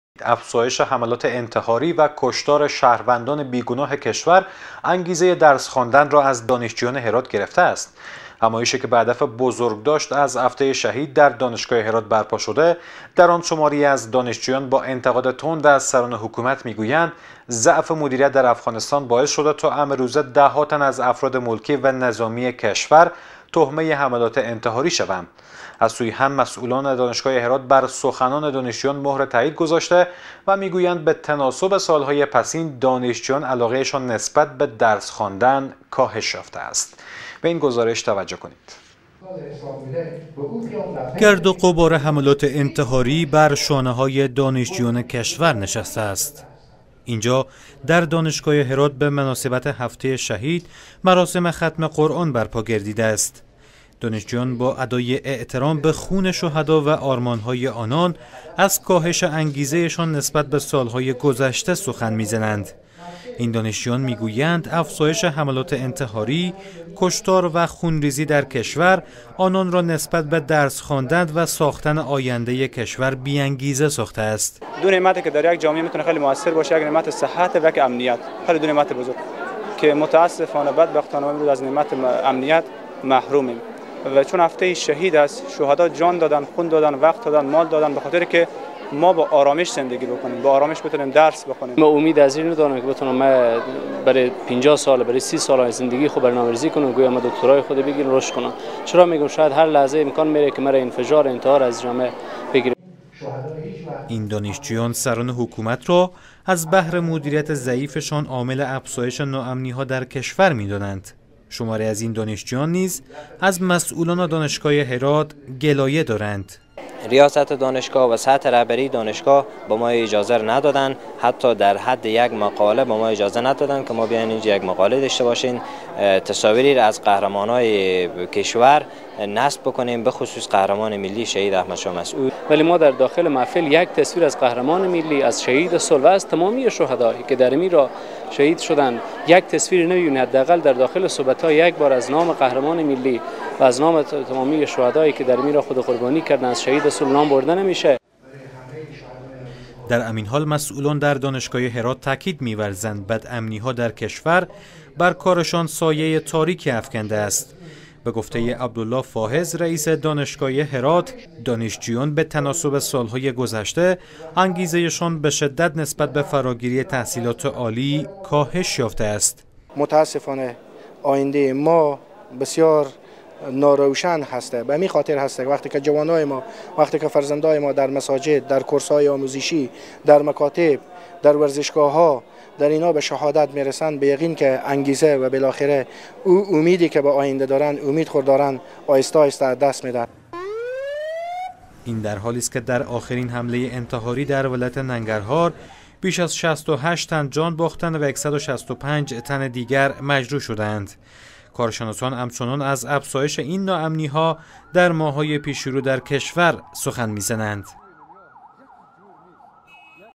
نگرانی دانشجویان دانشگاه هرات از افزایش حملات انتحاری و کشتار شهروندان بیگناه در کشور
افزایش حملات انتحاری و کشتار شهروندان بی گناه کشور، انگیزه درس خواندن را از دانشجویان هرات گرفته است